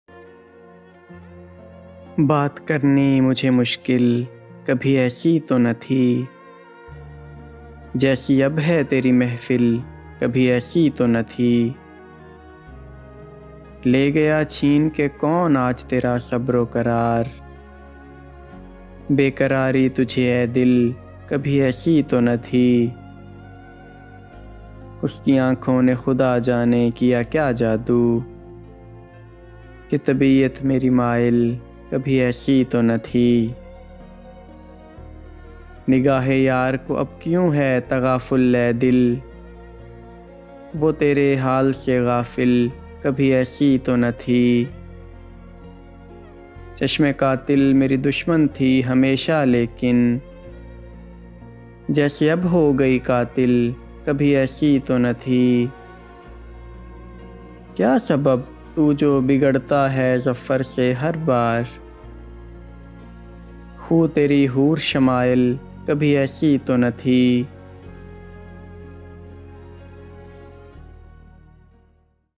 Baat Karni Mujhe| Bahadur Shah Zafar Poetry| By Jahan-e-Zouq
In this video you will listen to outclass poetry by famous Poet of Urdu Language.

بات کرنی مجھے مشکل کبھی ایسی تو نہ تھی
جیسی اب ہے تری محفل کبھی ایسی تو نہ تھی

Poet: Bahadur Shah Zafar

Narrator: Zia ur Rehman

Label: Jahan-e-Zouq